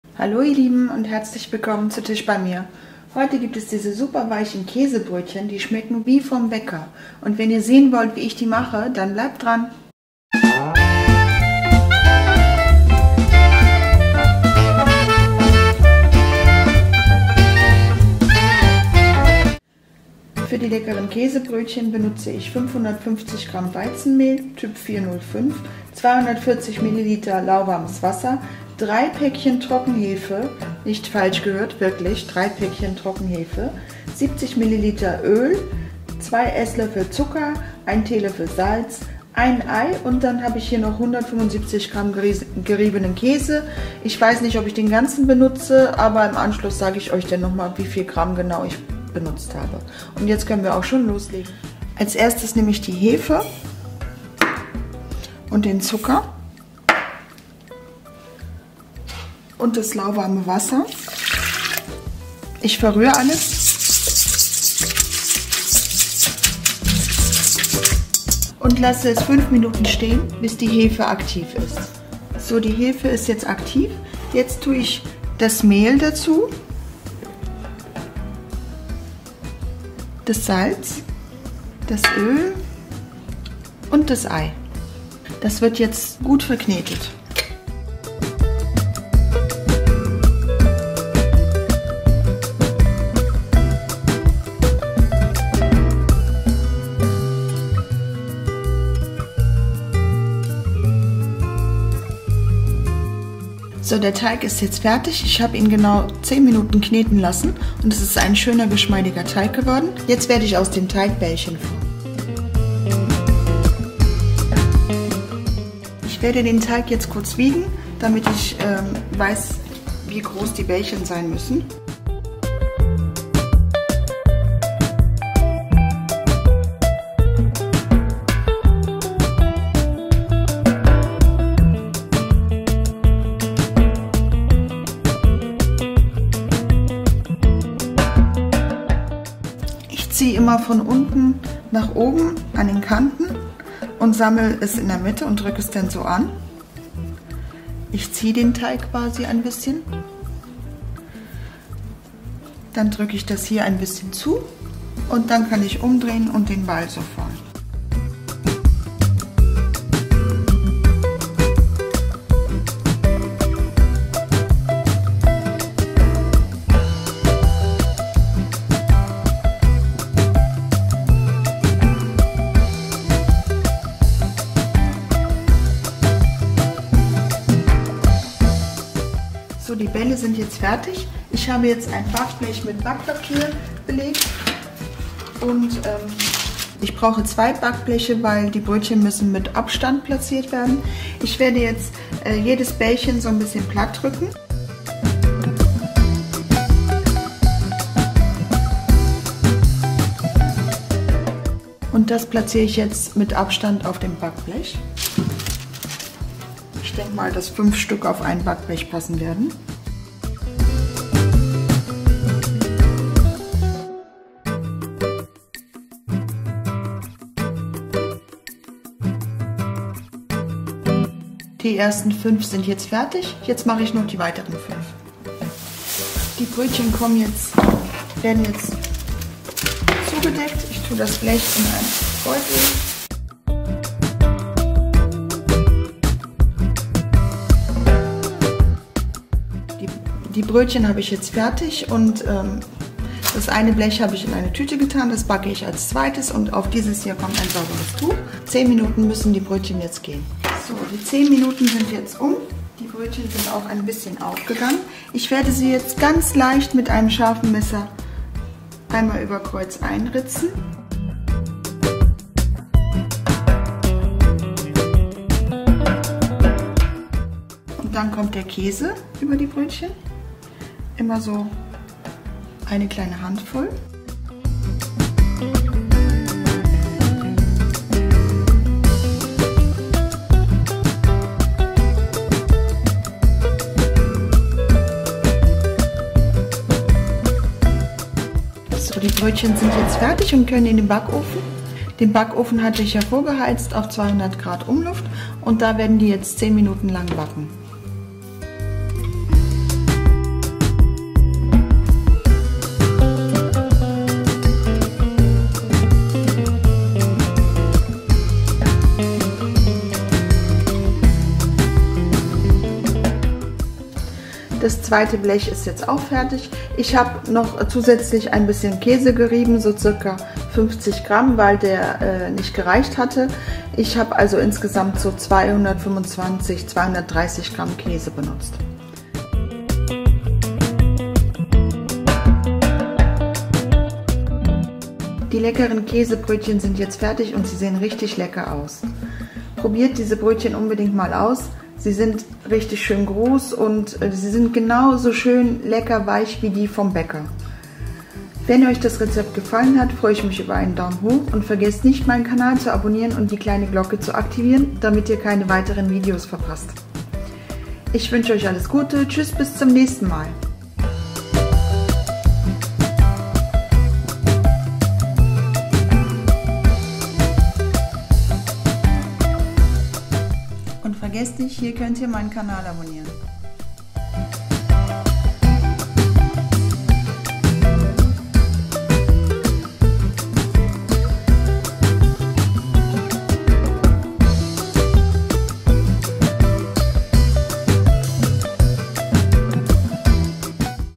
Wie vom Bäcker‼️Super weiche KÄSEBRÖTCHEN🧀💯Schnell gemacht ✅
Diese leckeren, weichen mit Käse überbackenen Brötchen die es beim Bäcker gibt, sind überhaupt nicht schwer zu machen. Ganz im Gegenteil! Innerhalb von kurzer Zeit sind die Brötchen auf dem Tisch.
Ich nehme auch sehr gerne weiche Brötchen für Sandwiches und dafür sind diese Brötchen Perfekt!
Probiert sie unbedingt mal aus!
Hier sind die Zutaten für das Rezept:
🔹240ml Wasser
🔹550g Weizenmehl (TYP 405)
🔹3 Päckchen Trockenhefe
🔹70ml Öl
🔹1 Ei
🔹225-230g gerieben Käse
Kitchenaid Küchenmaschine*
Ähnlicher Messbecher*

Filiz